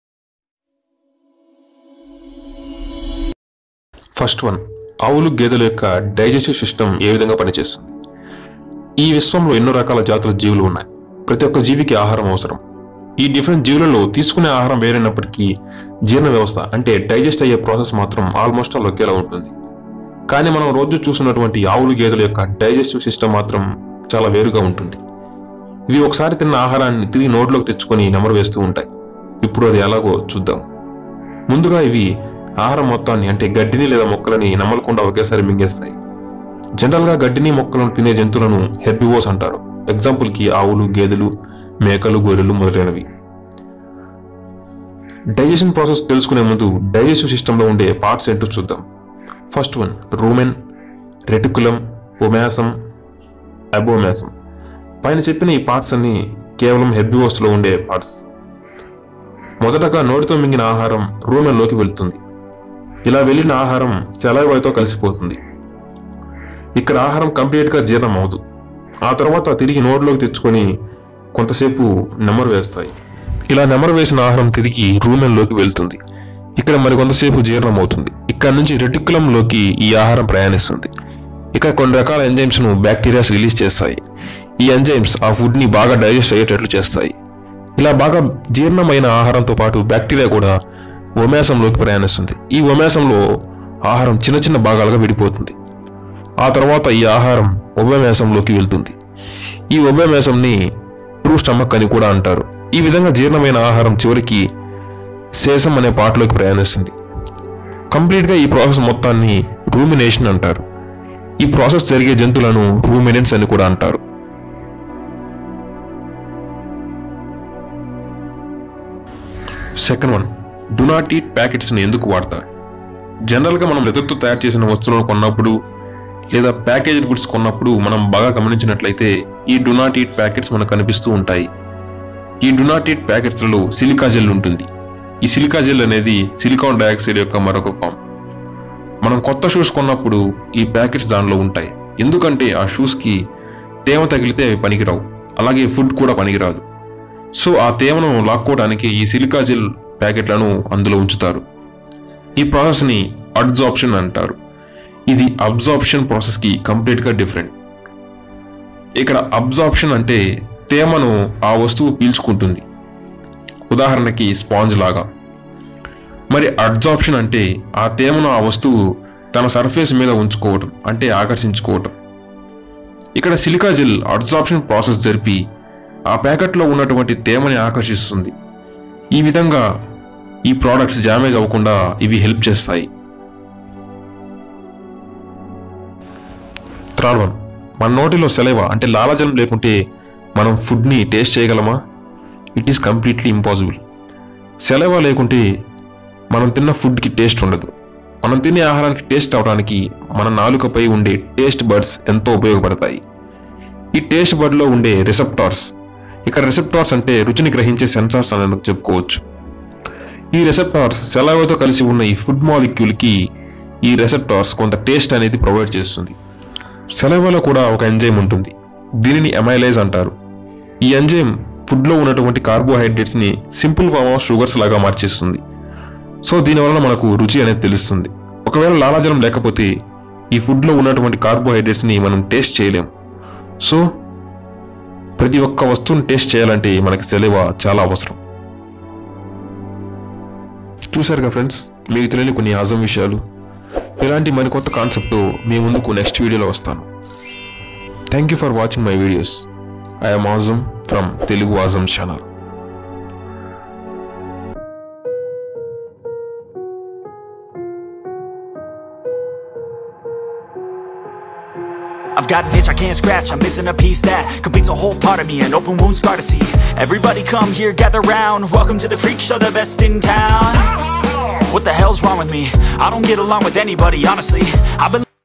How cows , Buffalos digestive process work | use of "Do not eat" packets | need of saliva | awesome
Hii viewers .
Sorry to say this , some where in video you won't listen my voice (only  bgm) due to copy right claim on the audio which I have used earlier . So please try to understand
Today we are going to see the interesting topics called .
1.How does cows , Buffalo s digestive system works
2. Why they provide "do not eat " packets in food preservatives ..And shoes boxes
3. Can we taste any food with out saliva ...??
Notice
 COPY RIGHT NOTICE:

If u like our videos .
TELUGU AWESOME CHANNEL